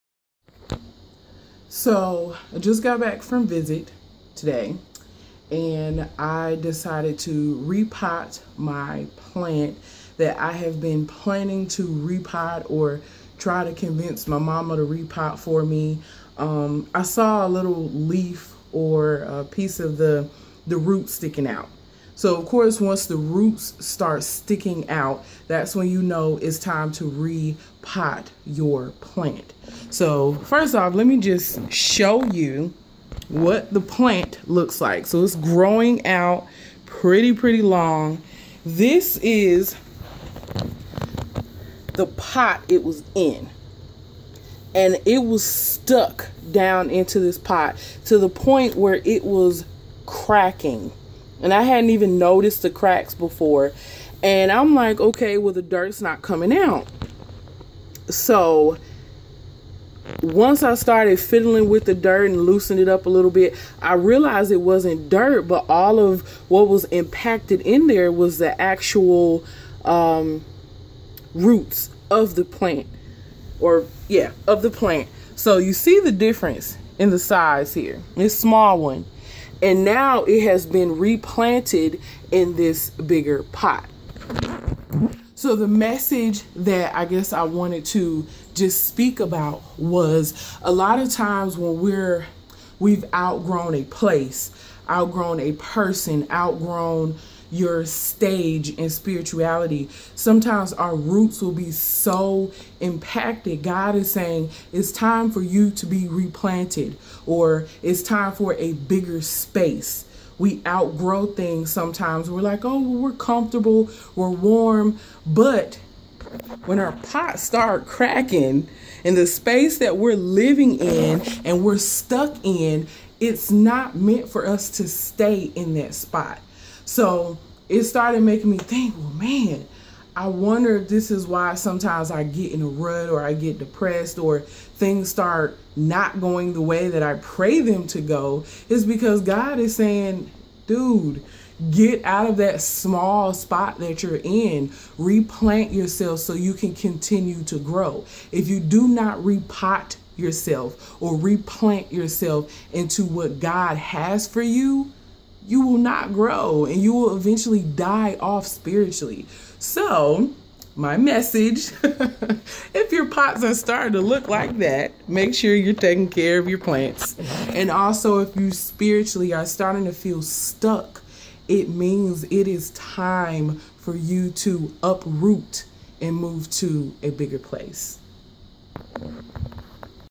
God Wants To Uproot And Replant You
Had to repot my plant today. It was outgrowing its small pot. Made me think if maybe God needs to replant me somewhere. It's really uncomfortable in tight spaces and we have to get up out of the comfort zone to reroot into something bigger to continue to grow.